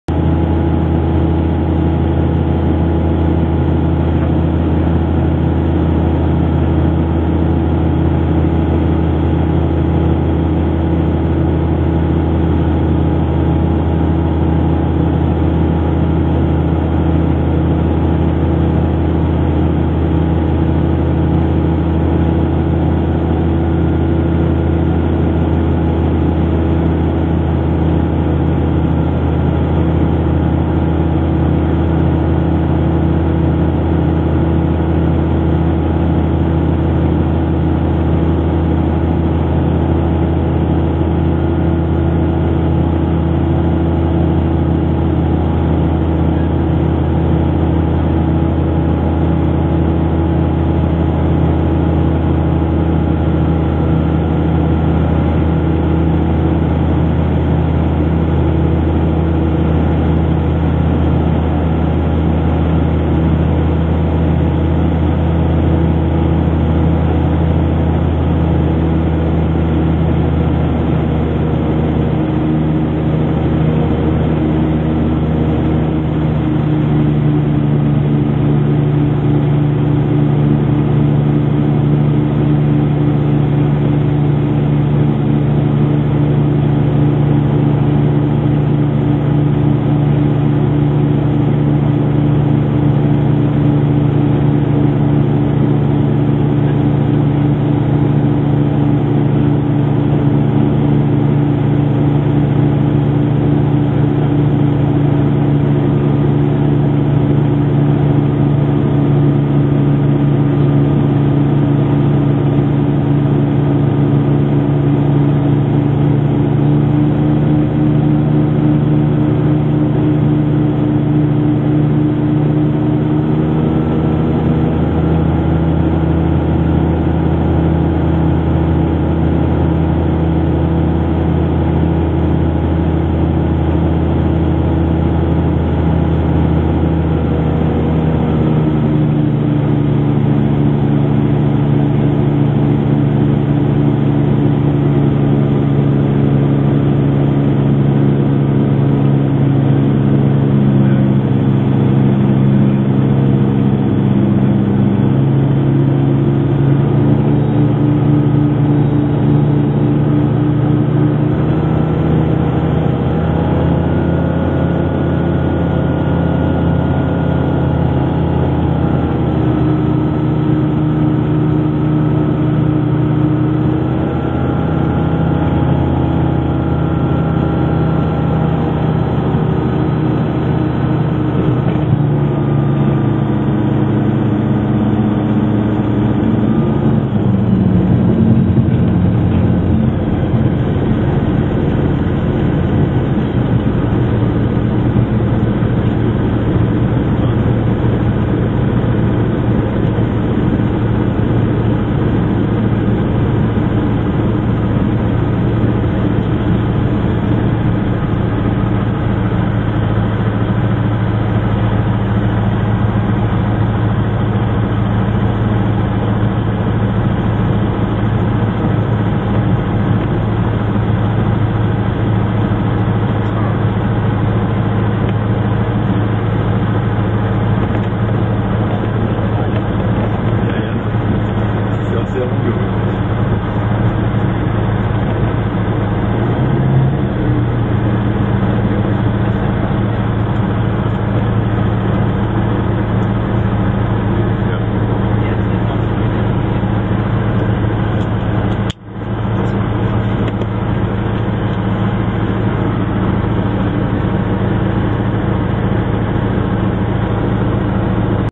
Landeanflug Gyrocopter Xenon 4 Flugplatz St.Peter-Ording 360° Cockpit View
Tragschrauber|Gyrocopter Xenon 4 Landeanflug und Landung auf dem Flugplatz St.Peter-Ording. In 360° aufgenommen mit der Kodak Pixpro 360°.